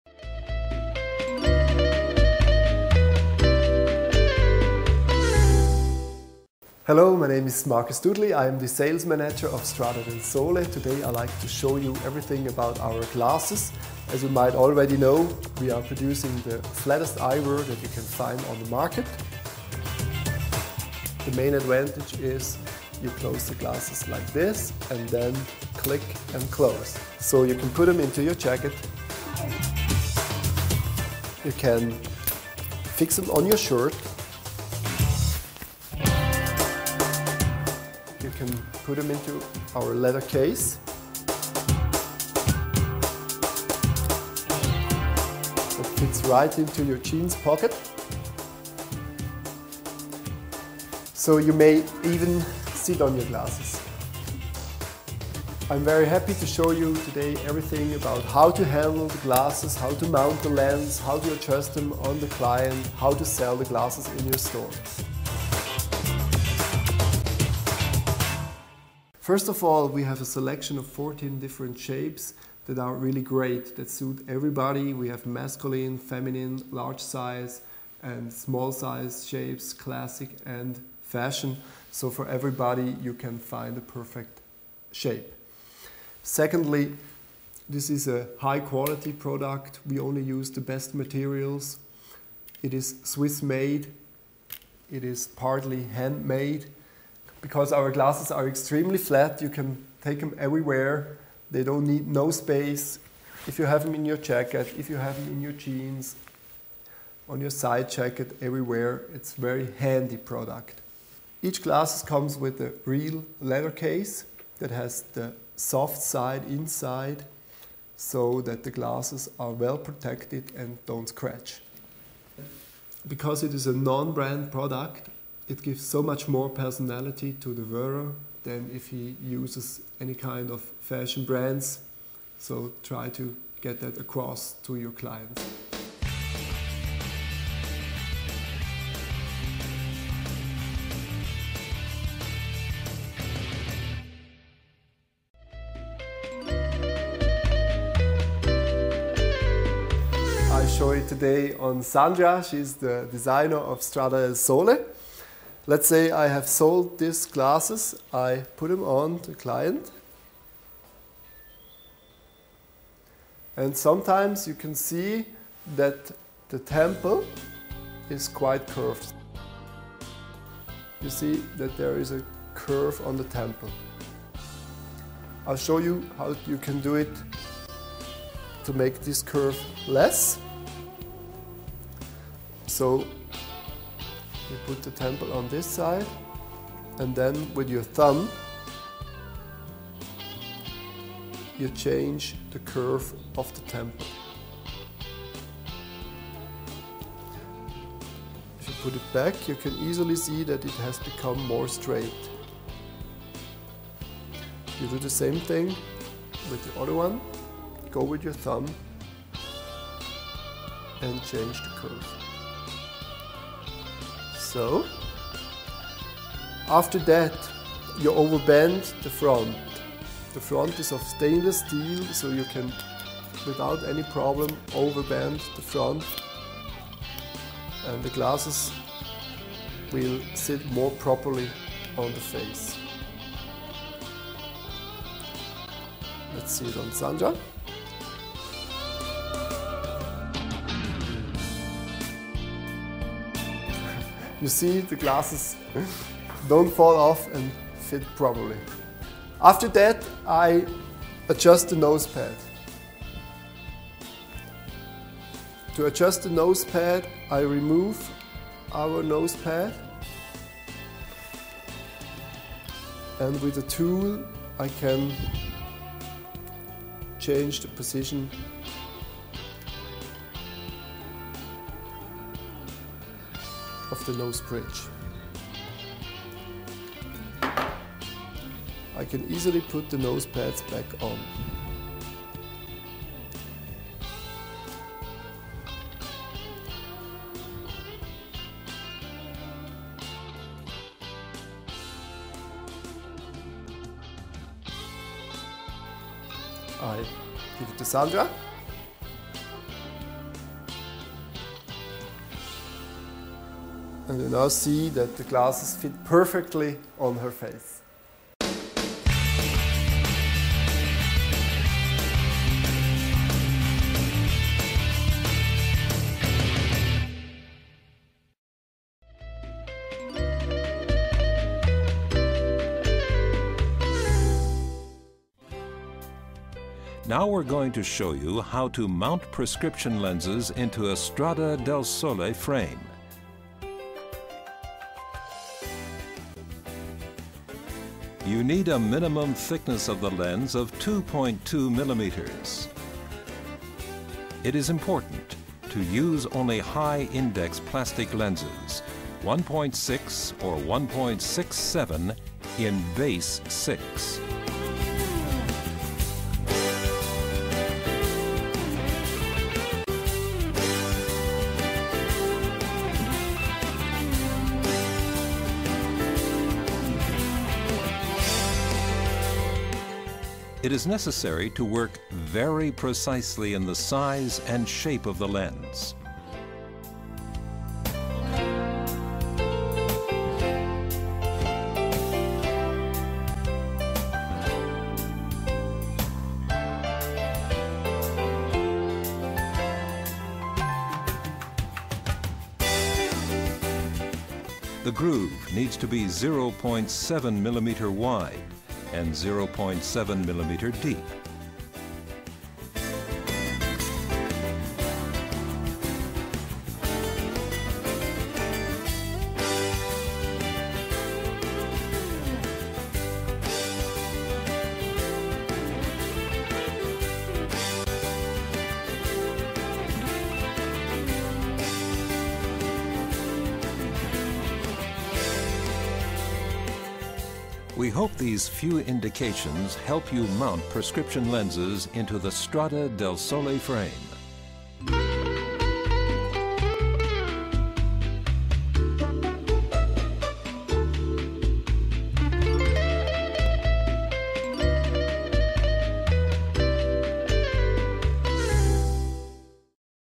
Strada Del Sole
The Swiss collection with the unique click mechanism: almost by sleight of hand the glasses are folded .
completely flat and fit easily into a trouser pocket true to style , an eye-catching combination of clear design and high functionality.